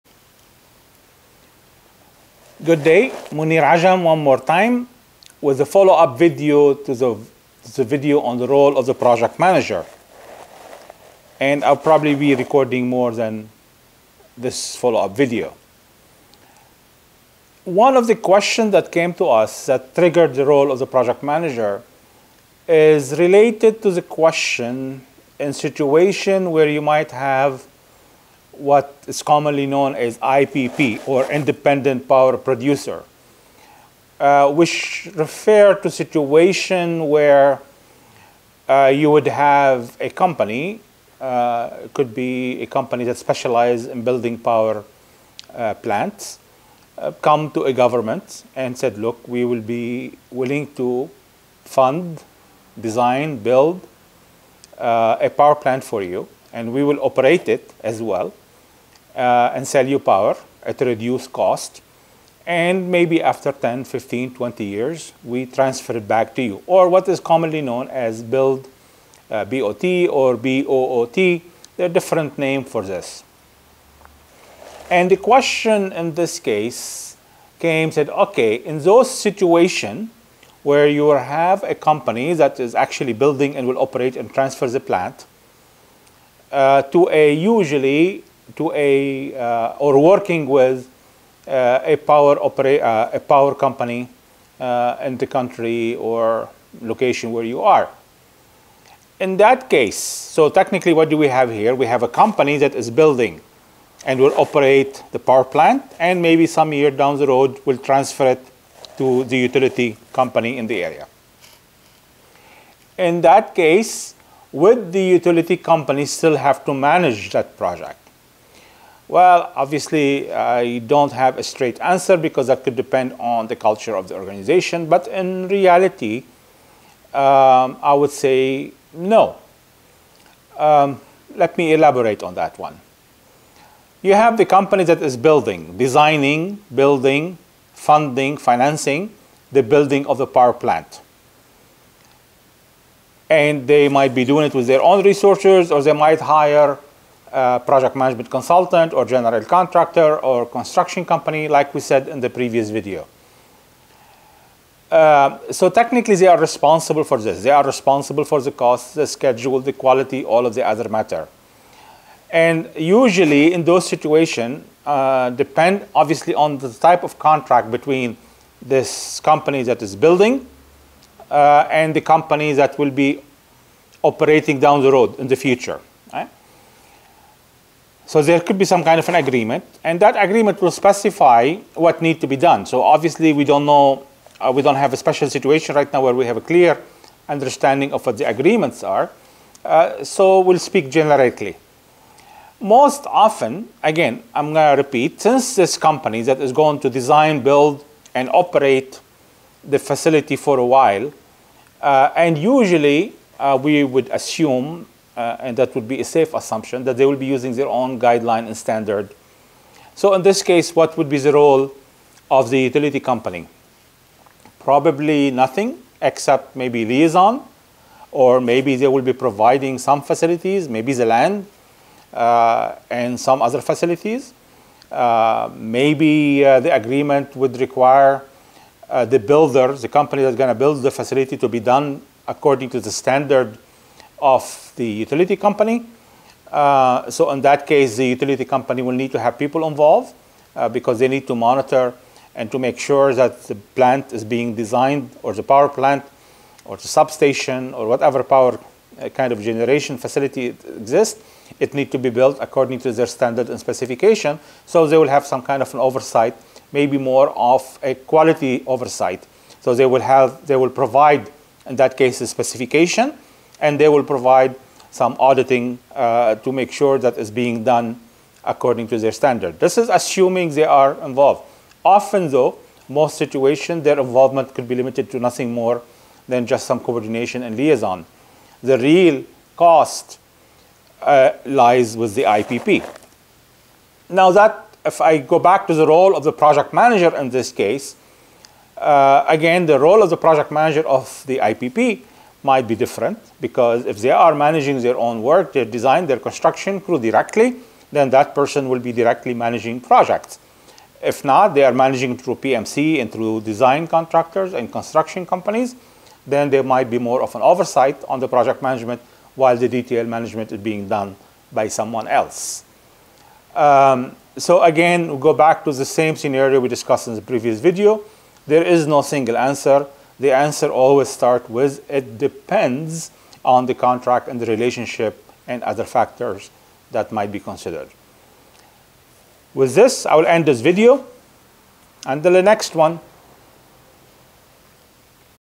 Do project owners still need a project manager in BOT situations?
If we have a BOT situation, does the project owner organization (utility, infrastructure org.) still need to have a project manager?